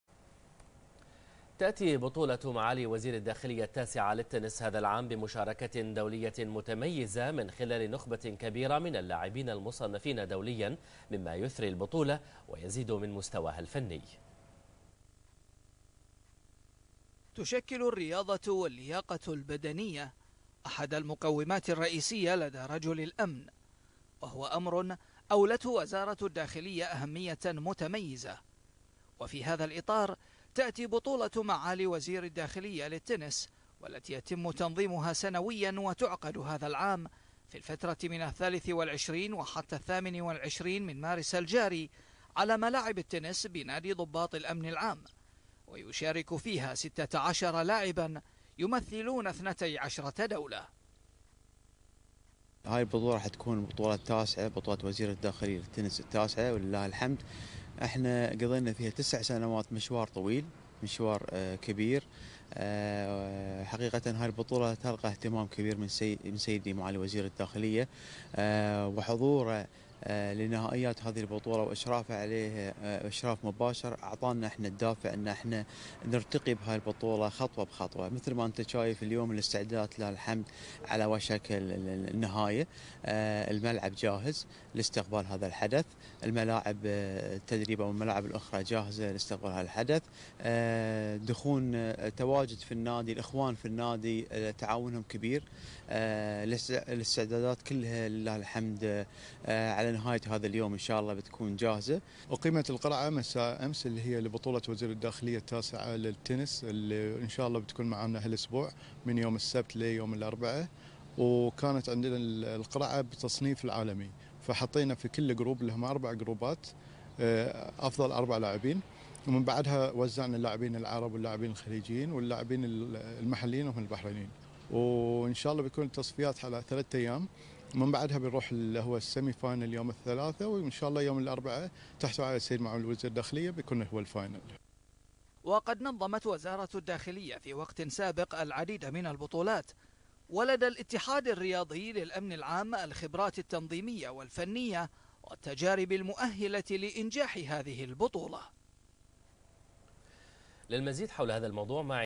مشاركة دولية متميزة في بطولـــة وزيـــر الداخلية المفتوحة التاســـعة للتنـس لسنة 𝟐𝟎𝟏𝟗
مشاركة دولية متميزة في بطولـــة وزيـــر الداخلية المفتوحة التاســـعة لسنة 2019

9th minister of interior tennis open championship 2019